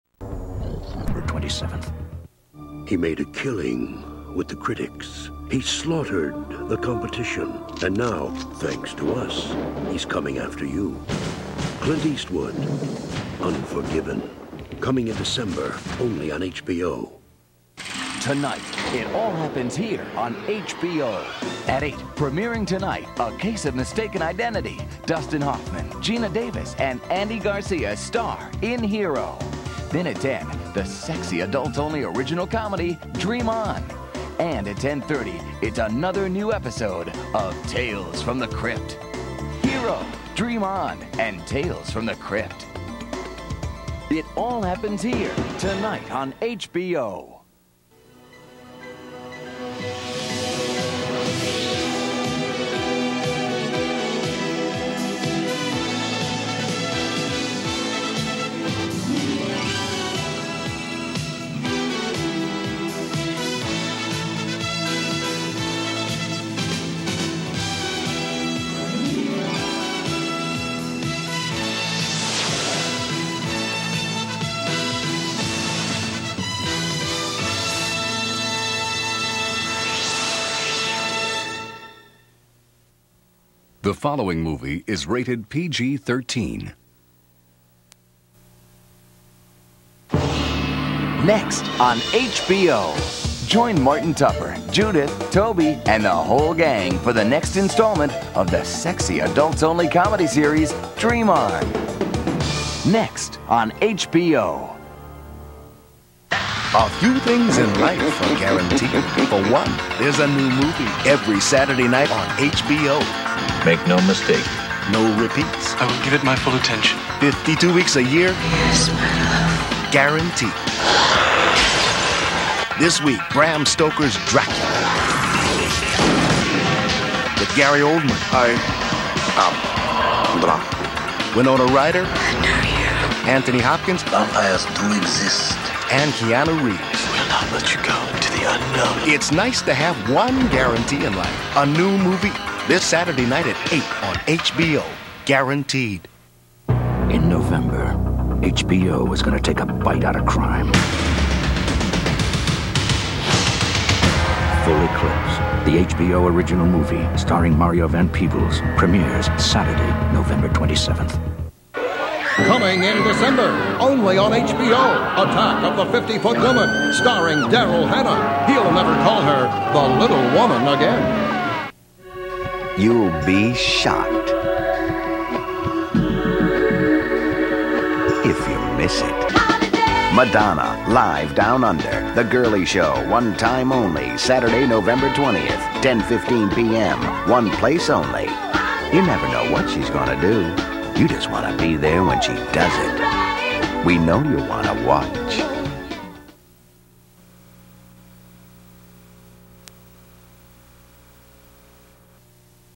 HBO promos [November 13, 1993]
Aired before and after Hero (1992).